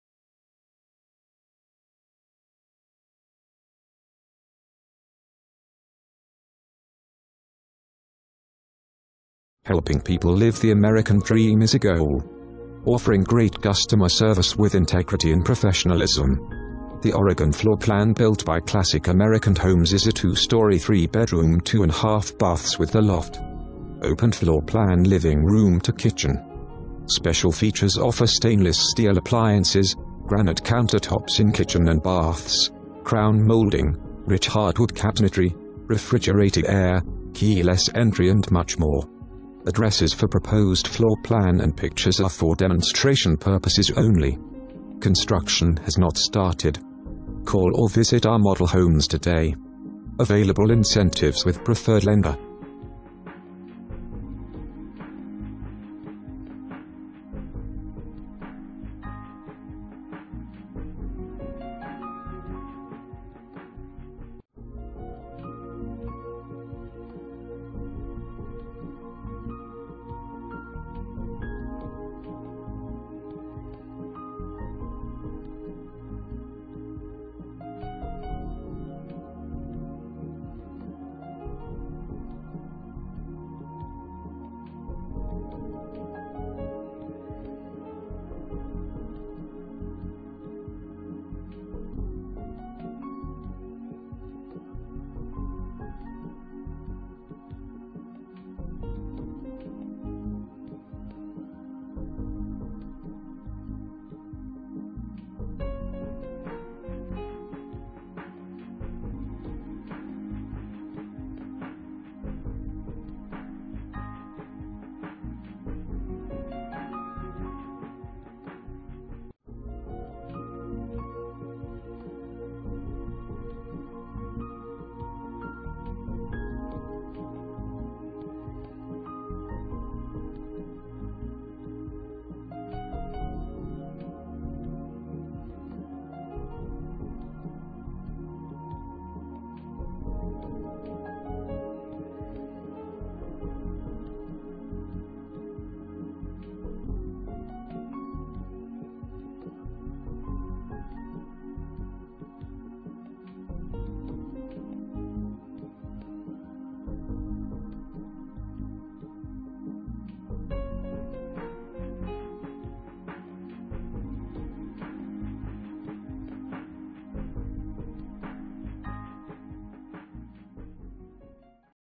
7816 ENCHANTED PATH Drive
The Oregon Floor Plan built by Classic American Homes is a two story three bedroom two and half baths with a loft. Open Floor Plan Living room to kitchen. Special features offer stainless steal appliances, granite counter tops in kitchen and baths, crown molding, rich hardwood cabinetry, refrigerated air, key-less entry and much more. Address is for proposed floor plan and pictures are for demonstration purposes only. Construction has not started. Call or visit our model homes today. Available Incentives with Preferred Lender.

Presented By:
Thelma Briffa, Better Homes and Gardens Real Estate ELEVATE